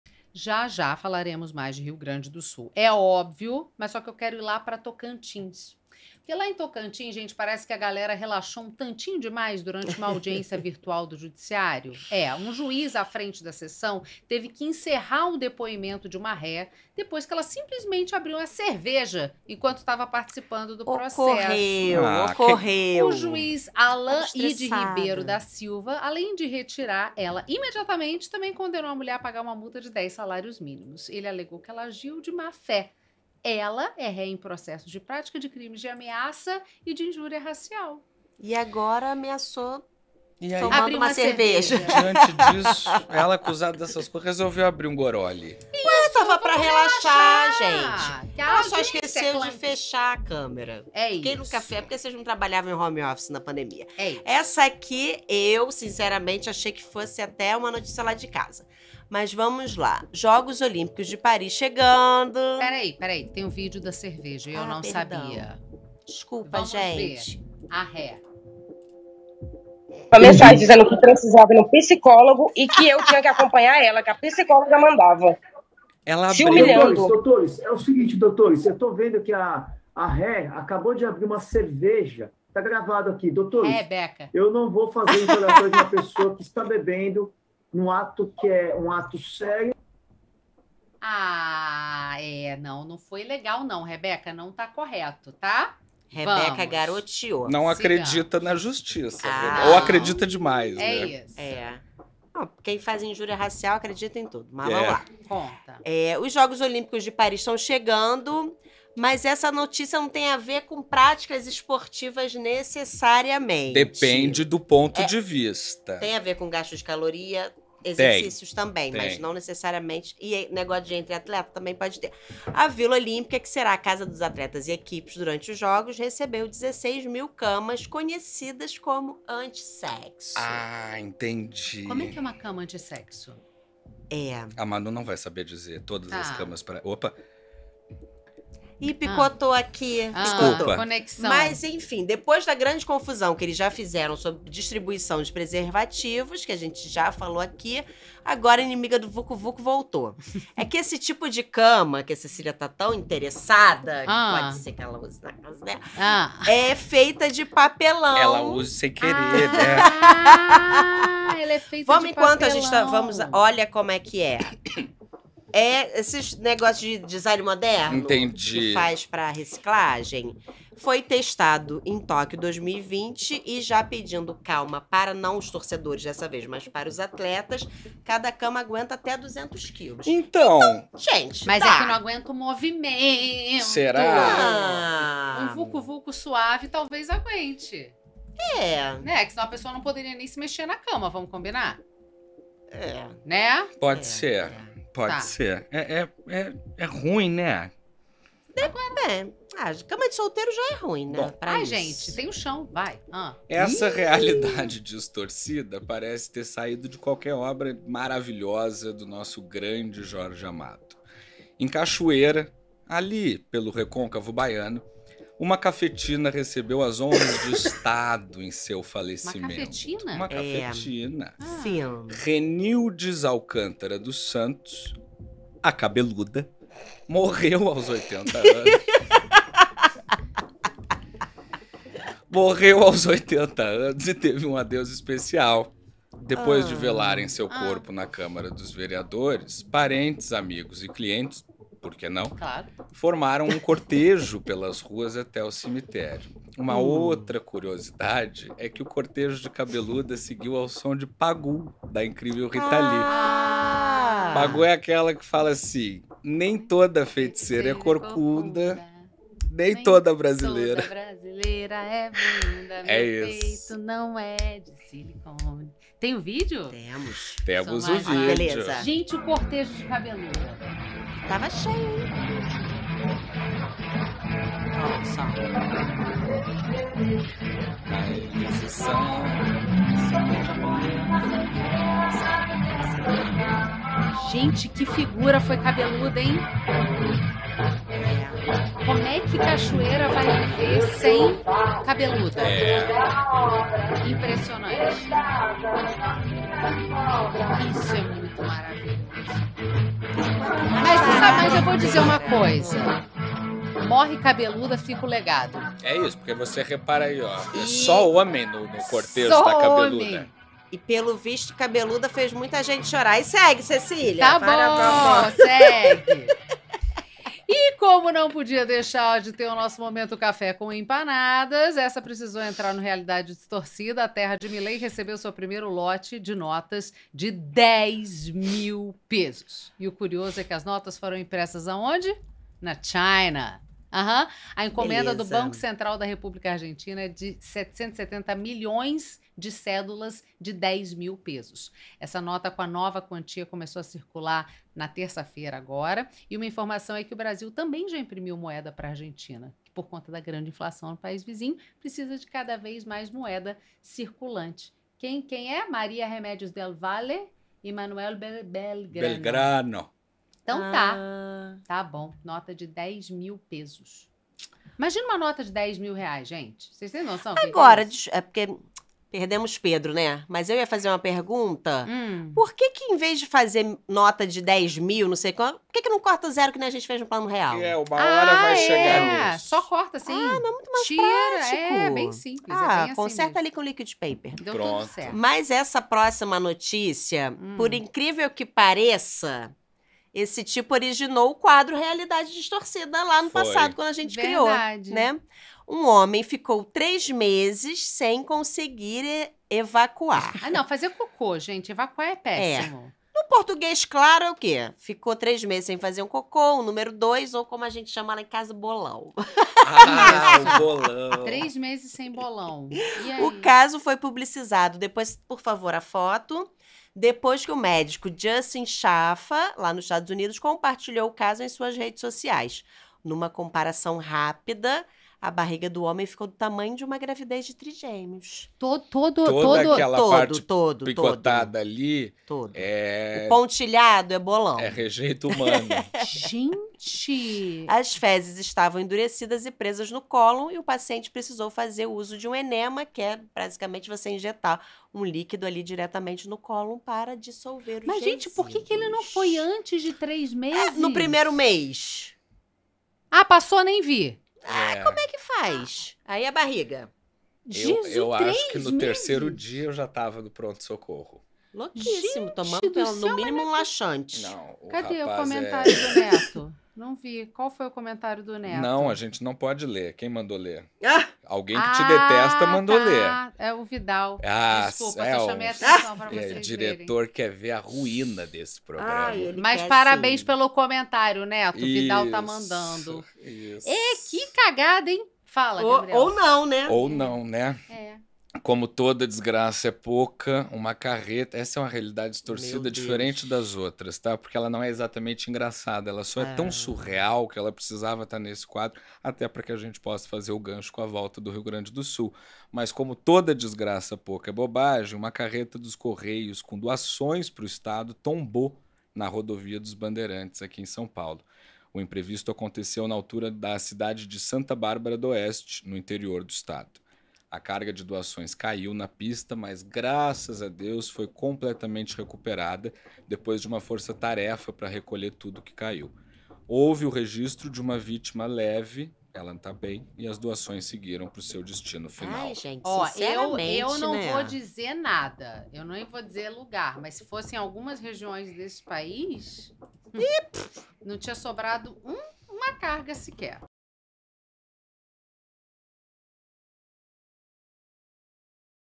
CAFETINA HONRADA, CERVEJA NA AUDIÊNCIA E HOMEM CONSTIPADO ENTRE AS NOTÍCIAS MAIS ABSURDAS DA SEMANA
Nosso querido quadro REALIDADE DISTORCIDA sempre traz as notícias mais bizarras e absurdas da semana, que parecem mentira mas são a mais pura verdade!

Na edição dessa semana temos: uma ré abrindo uma cerveja no meio de seu julgamento, as honras de estado dadas à cafetina Dona Cabeluda após sua morte (que descanse em paz), o homem que passou três meses sem ir ao banheiro para fazer o número 2, a nova nota de 10.000 pesos na Argentina (dez mil!) e as camas anti-s3xo recebidas em Paris para as Olímpiadas que serão sediadas lá!

Assista e nos diga nos comentários qual dessas notícias você achou mais absurda!